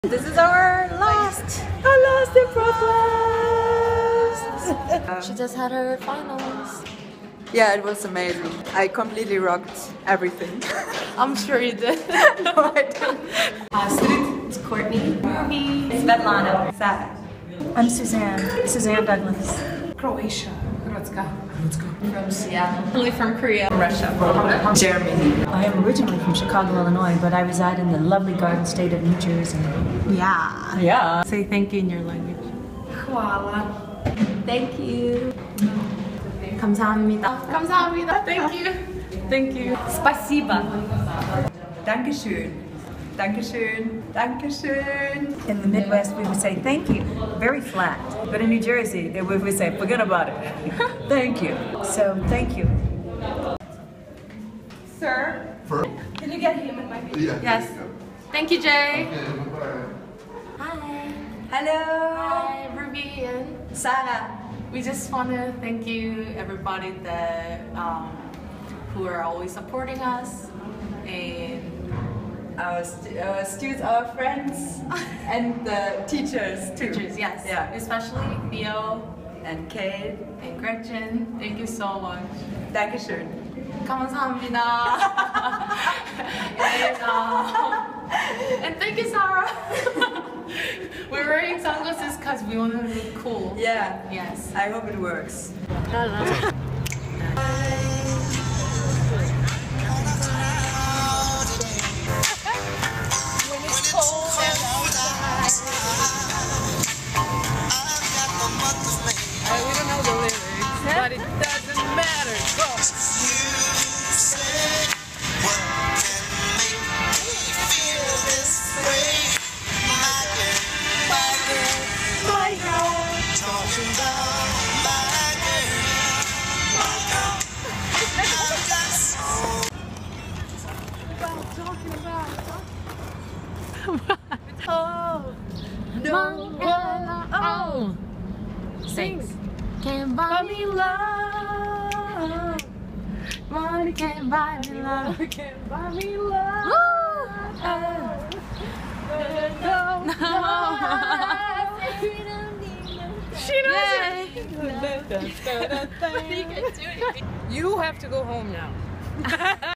Manhattan School of Music Jazz Vocals 맨하탄 음대 재즈 보컬 2015 🎵
filmed and edited by Ruby
just another crazy day at school with my ladies xx

Singers :

Suzzanne Douglas
Astrid Kuljanic
Svetlana Shmulyian
Ruby Choi
Courtney Cutchins
Sara Decker

Special Thanks To :

Theo Bleckmann
Kate McGarry
Gretchen Parlato

Jay Anderson
Yoon Hwa Choi
Eitan Gofman
Eunhye Oh
Melissa Panlasigui

Locations :

Manhattan School of Music
Sakura Park

just in case @1:37 ~ ;
drinking Iced Tea, not Beer LOL

‪#‎msmjazzvoiceclassof2015‬